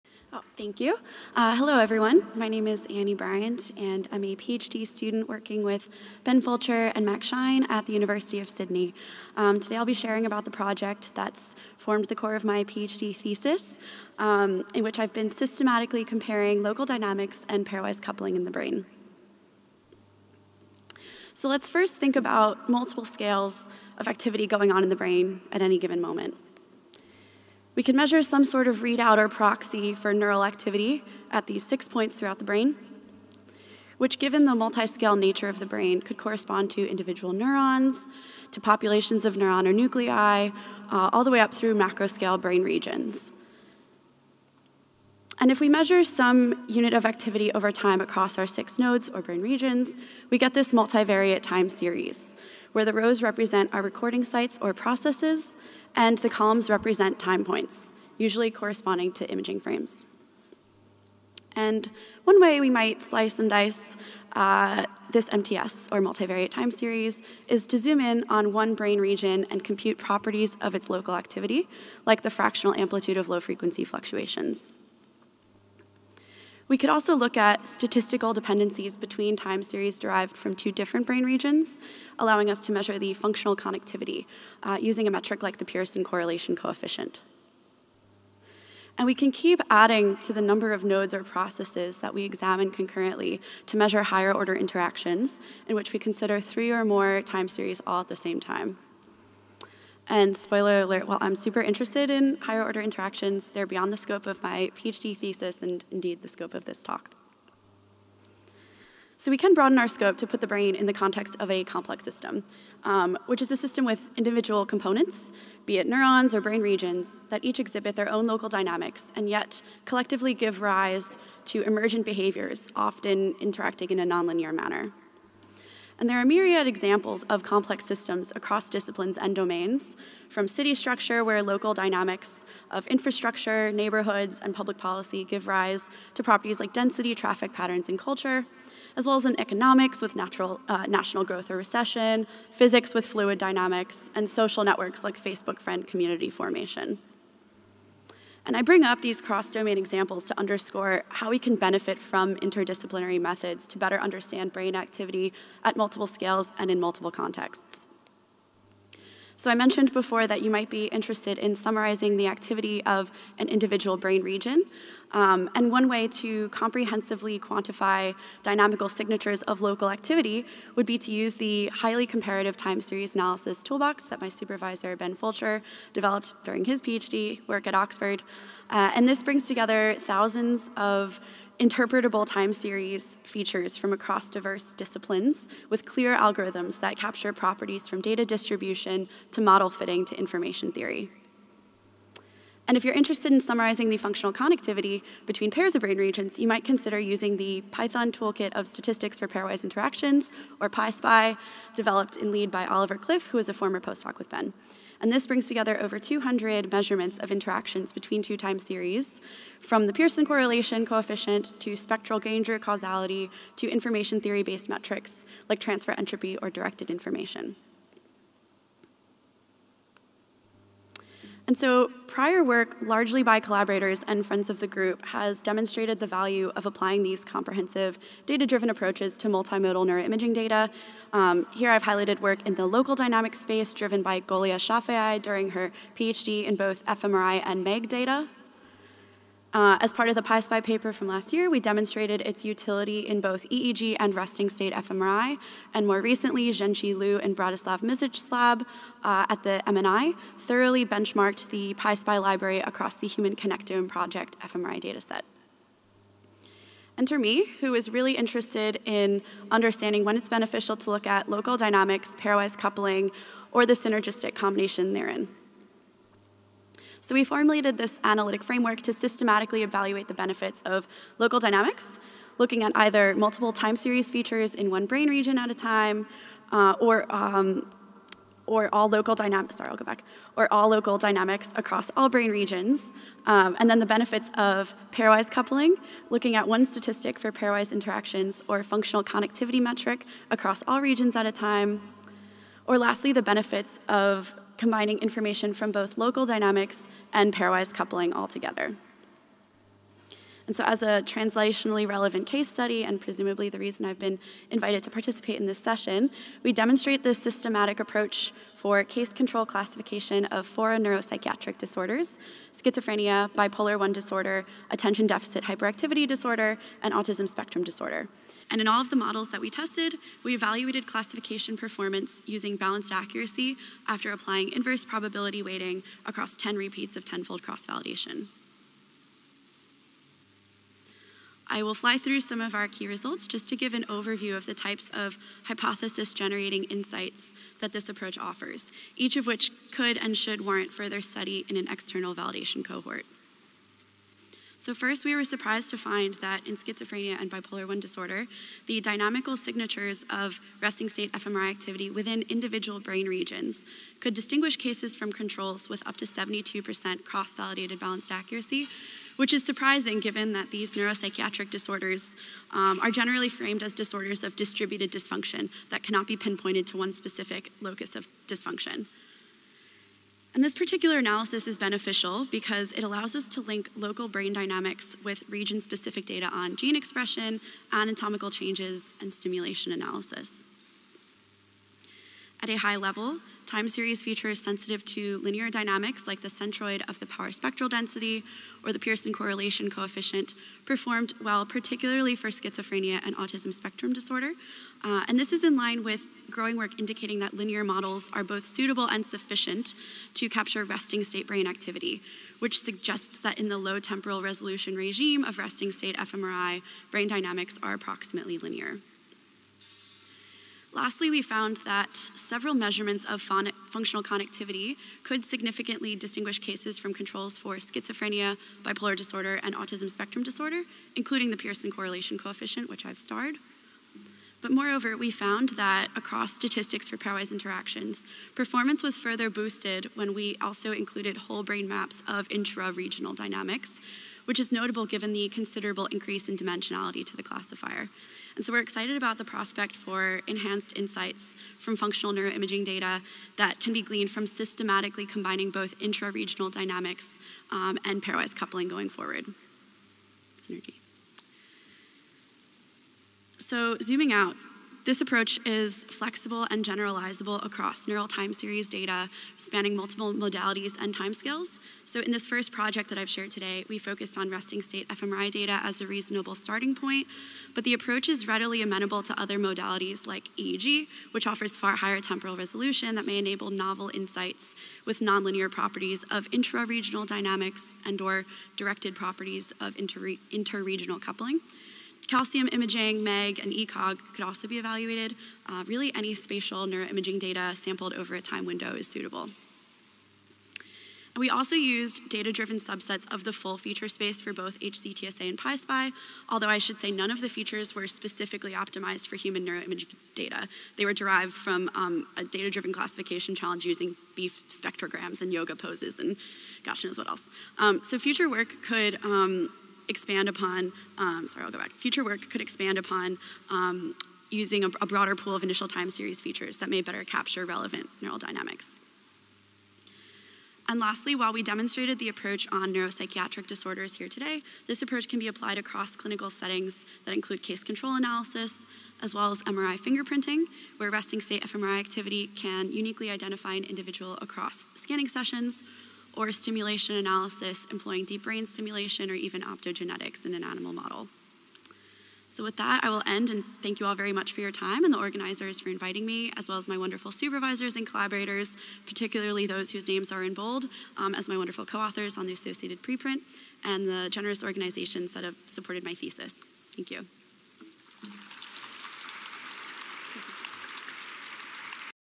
OHBM 2024 | Oral Session | Annie Bryant | Systematically comparing properties of local dynamics a…
OHBM 2024 Oral Session
Session: Transdiagnostic Perspectives on Neurodevelopmental and Psychiatric Disorders - Part 1
Part 5
Title:  Systematically comparing properties of local dynamics and pairwise coupling in the brain.
Speaker: Annie Bryant
Abstract: Dynamical structures of brain activity can be quantified from functional magnetic resonance imaging (fMRI), like local regional activity and functional connectivity (FC) between pairs of regions (Fig 1A). To date, most studies use only one of the two representations with a limited set of statistics, like the fractional amplitude of low-frequency fluctuations (fALFF) for regional dynamics and the Pearson correlation coefficient for FC [1]. Emerging work using comprehensive libraries of interdisciplinary time-series features [2,3] suggests that alternative statistics may be more suitable for a given application [3,4], though there is currently no unifying framework for comparing across multiple features and representation types simultaneously. Here, we introduce a systematic approach to quantify diverse types of local and pairwise dynamical structure from fMRI data, comparing the ability of multiple feature-based representations to capture meaningful differences in neuropsychiatric casecontrol datasets.

Problem with this video? Is the speaker or talk title incorrect or unmatched with the video?